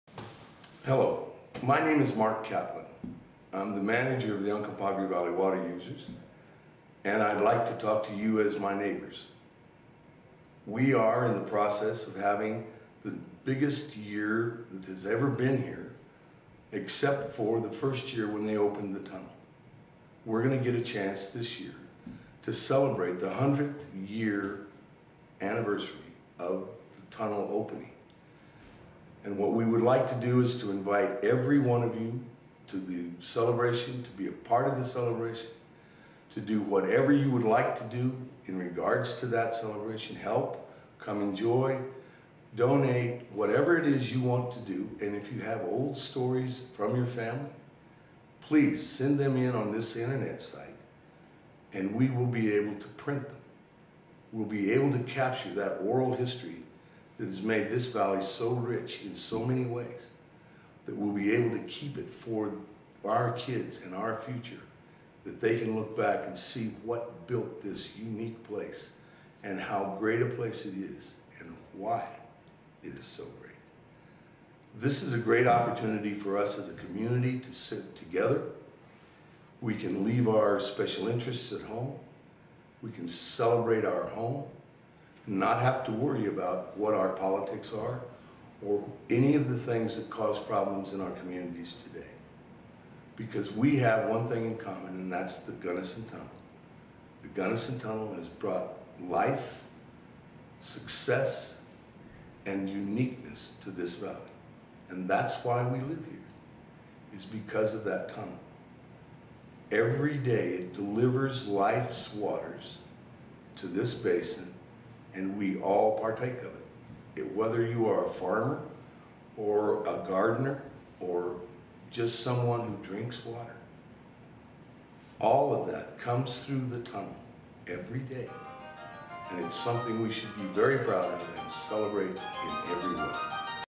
Welcome to the 100 Years of Water celebration by Marc Catlin
Marc Catlin, manager of the Uncompahgre Valley Water Users Association in Montrose, Colorado, welcomes everyone to the 100th anniversary celebration of the Gunnison Tunnel.